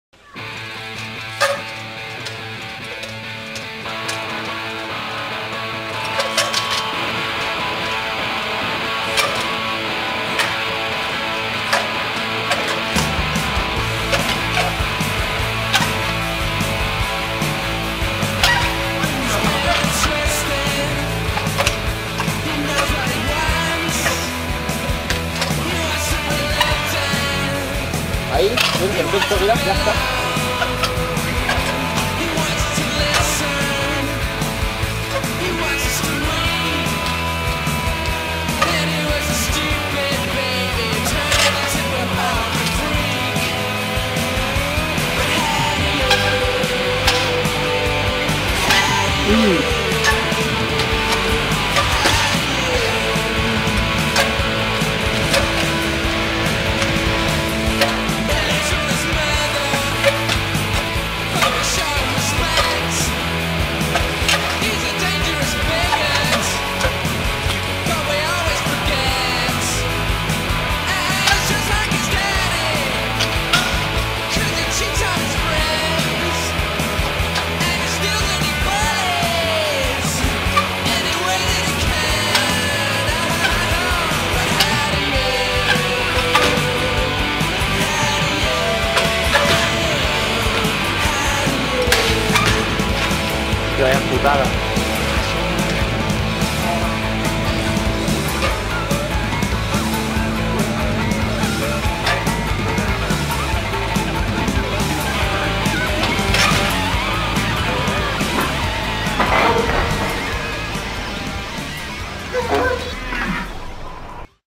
anticlips lloret
Entrenillo con Ivan AntiSpotSearcher xD y un servidor.. En Lloret city.. La verdad es k es todo bastante peste pero luego nos marcamos un natty RocaGrossa y estubo genial (en breve haremos otro y grabaremos a muerrrrrte :P)